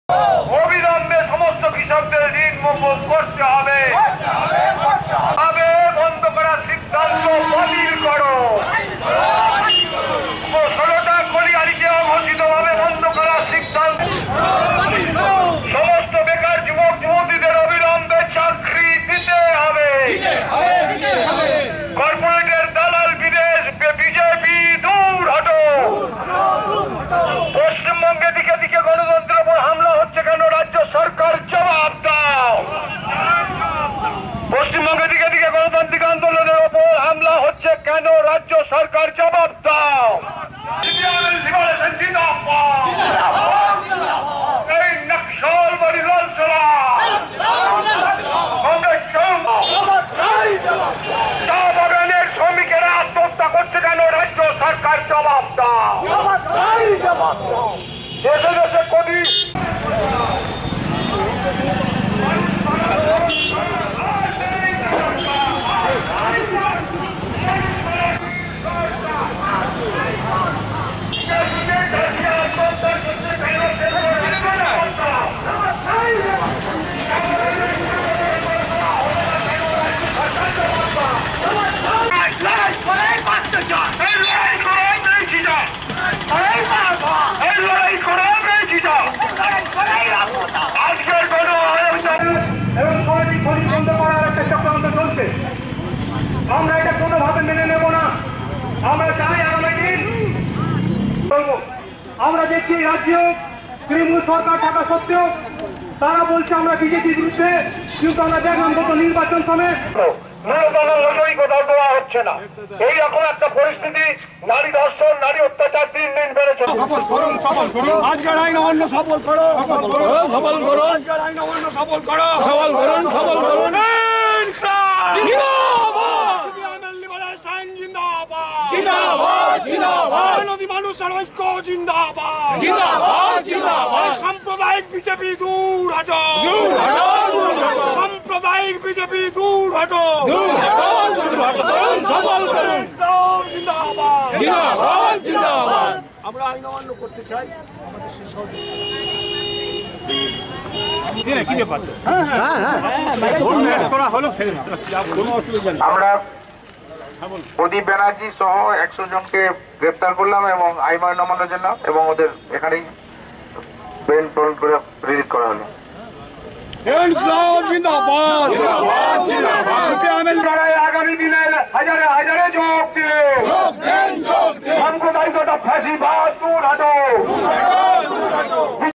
সিপিআই (এম এল) লিবারেশনের আইন অমান্য কর্মসূচী
আসানসোলে সিপিআই (এম এল) লিবারেশনের আইন অমান্য কর্মসূচী